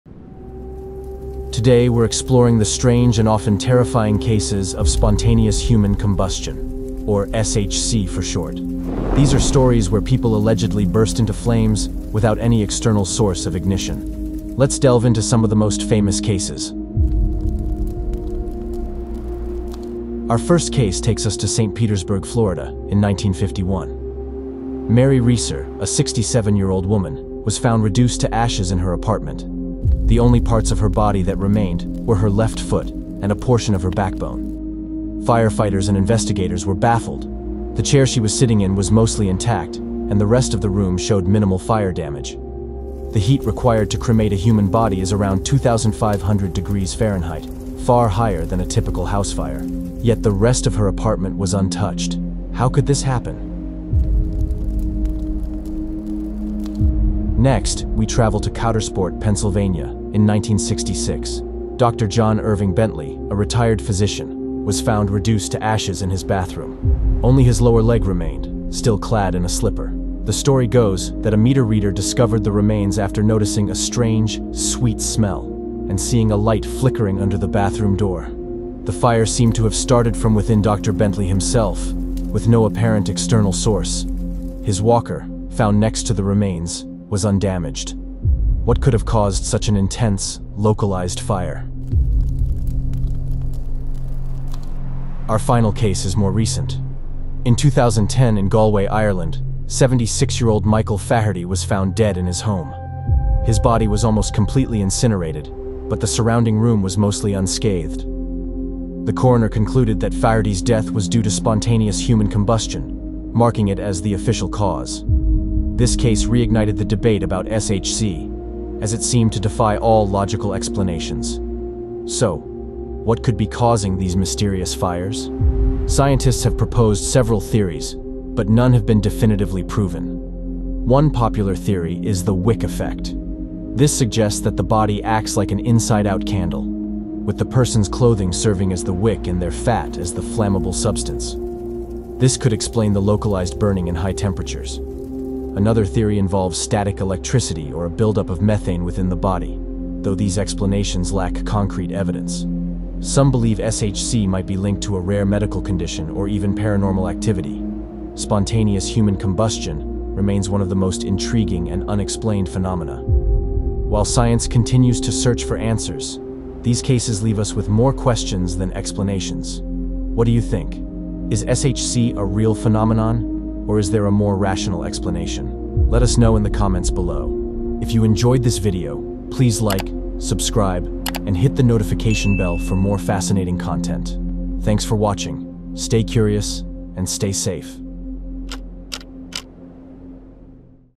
Spontaneous Human Combustion: Real Cases and Scientific Theories
Have you ever heard of spontaneous human combustion (SHC)? This mysterious and controversial phenomenon has puzzled scientists and intrigued the public for decades. In this video, we delve deep into the real cases, scientific theories, and myths surrounding spontaneous human combustion.